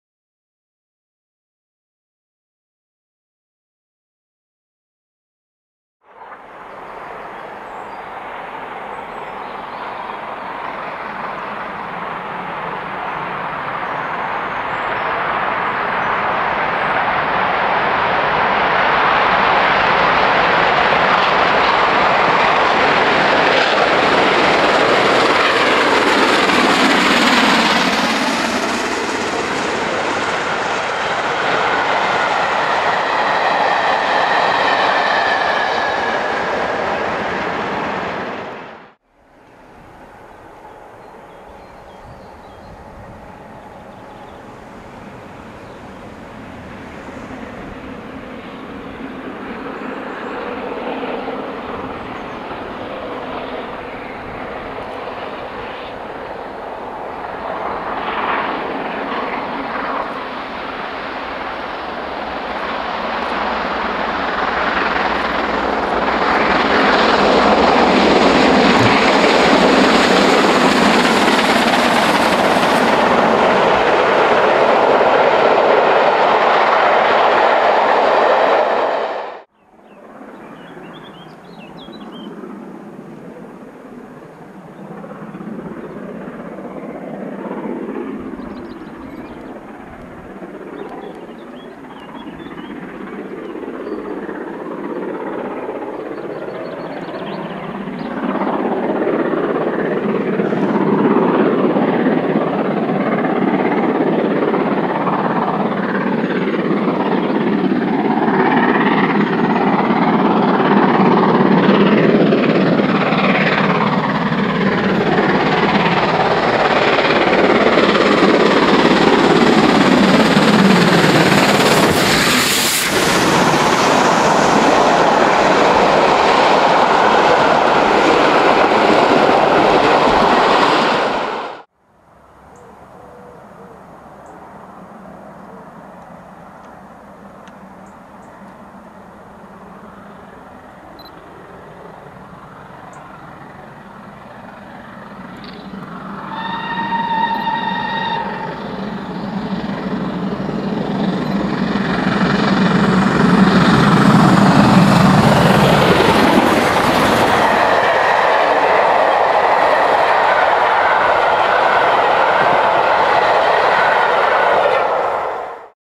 The Cornishman - 15th May 2010
The Cornishman - 15th May 2010 - with GWR 4-6-0s 4965 Rood Ashton Hall and 5043 Earl of Mount Edgcumbe.  Videos taken on the outward trip at Whiteball and Stoneycombe, on the return trip at Dainton and Tiverton Parkway.